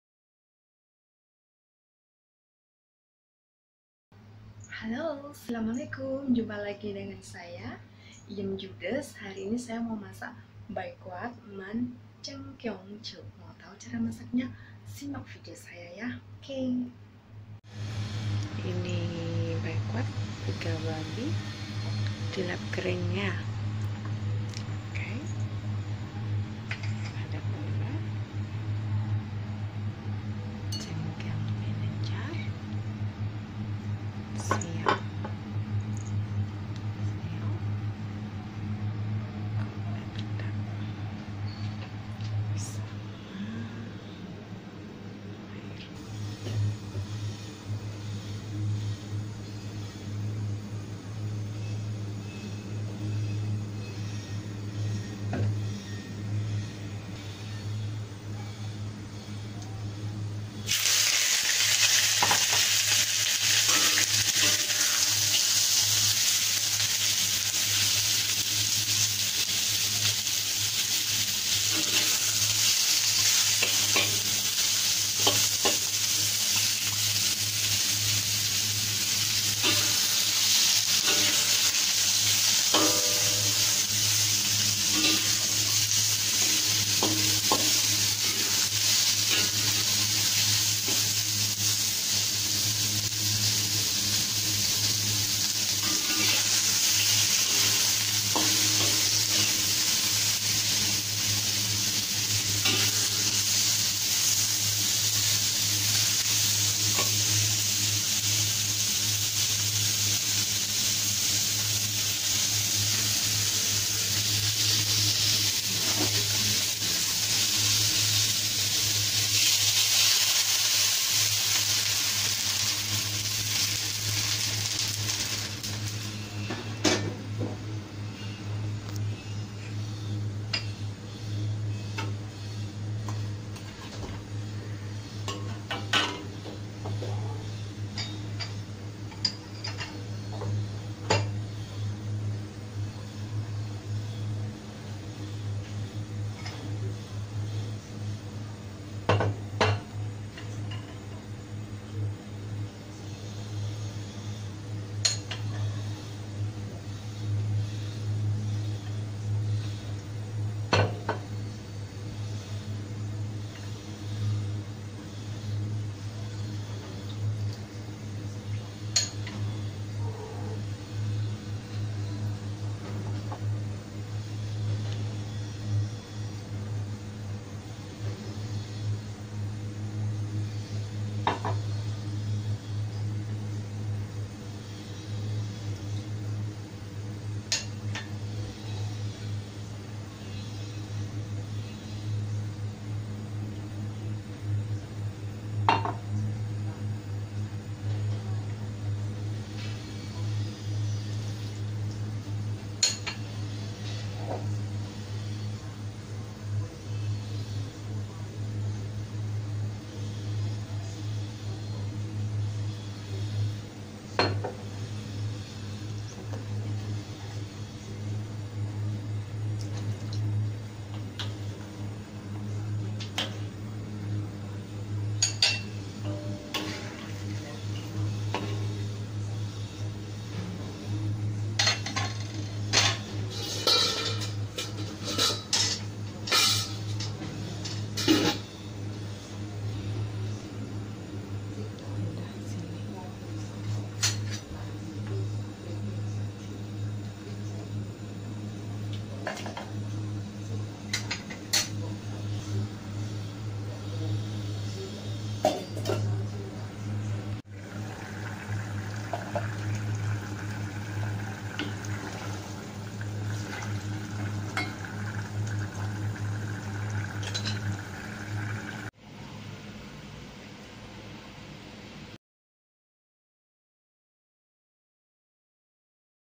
BAIKWAT MAN CENGKONG JO
BAIKWAT MAN CENGKIONG JO
(iga babi ungkep chingkiang vinegar)

Bahan:
Baikwat/ iga babi

Bumbu:
Cengkong jo/ chingkiang vinegar 2 sdm
Siyau/ kecap asin 2 sdm
Lojau/ kecap hitam 1 sdm
Bindong/ gula merah 1/3 batang
Mayau/ minyak wijen 1 sdm

Cara masak:
Baikwat cuci bersih lalu lap kering.
Panaskan wajan kasih minyak dikit, masukan baikwat, tumis kecoklatan, lalu masukan air banyak, masukan semua bumbu, rebus beruap lalu kecilkan api, masak 1/2 jam lalu matikan kompor tutup biarkan 1/2jam baru rebus lagi api kecil sampai empuk, airnya tinggal dikit.
(gunanya biar cepat empuk irit gas)
Silahkan mencoba ya. !!!
Kata bosku uenaaaaakkk.